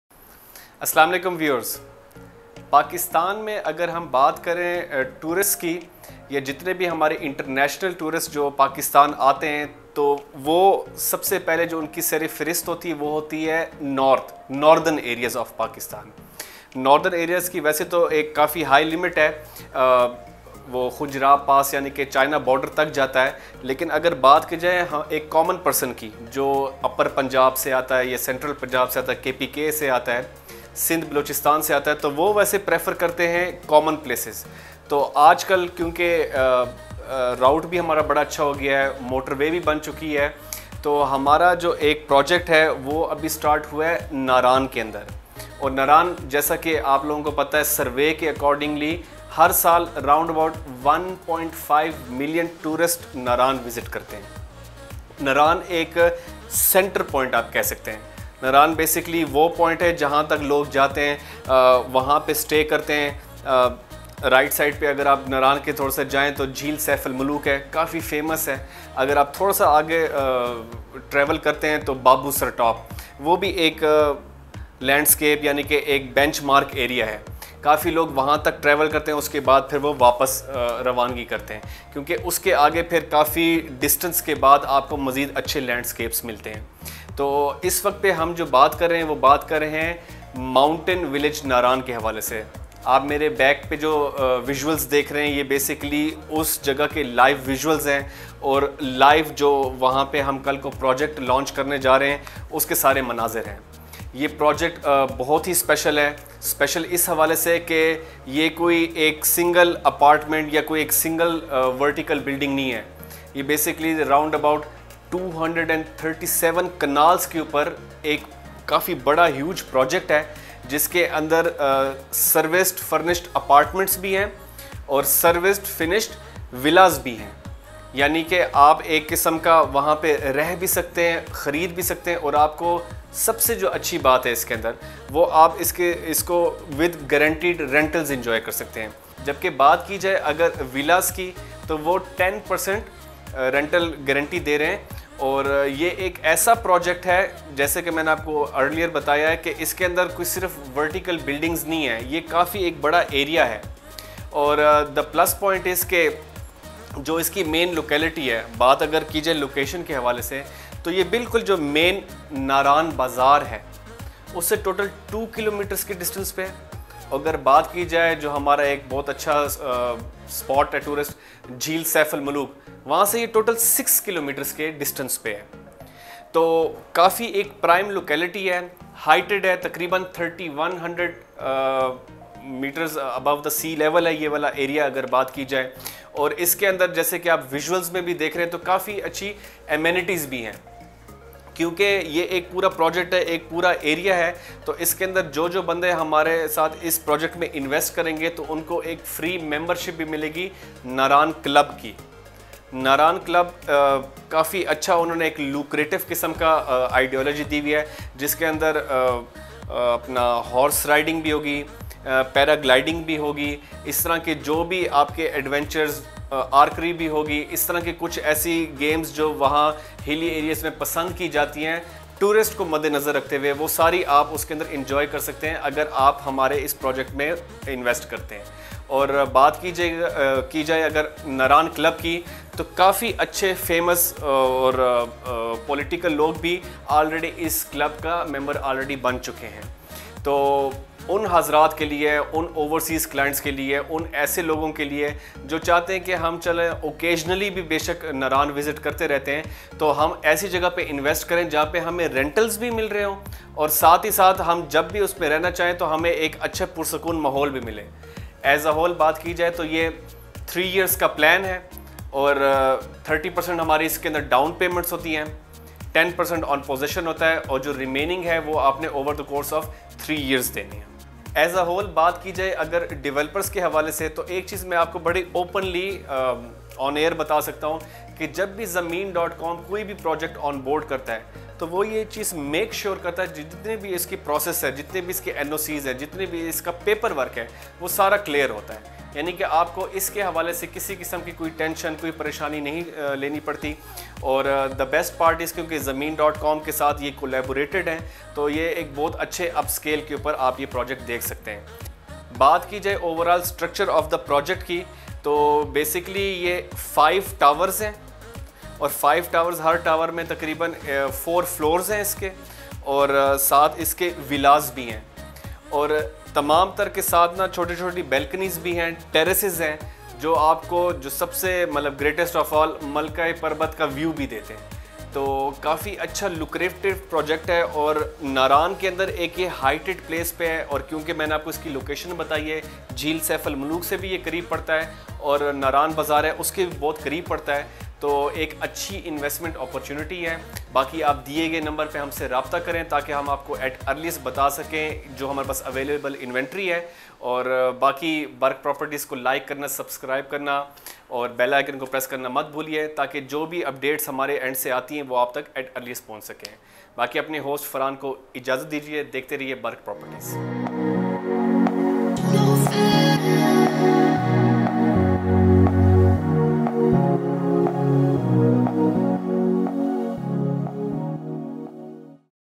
Mountain Village Naran - Shops, Apartments & Villas - Investment In Naran Kaghan
The Mountain Village Naran is a great Opportunity to Invest in your dream location and a getaway location. It could be your vacation house as well as your source of income.

In the heart of Naran Valley.
Few Minute's Drive from Naran Baraz Towards Lake Saif Ul Malook

Food Courts, Commercial Apartments, and Cottages are available at the most prime location of NARAN Valley.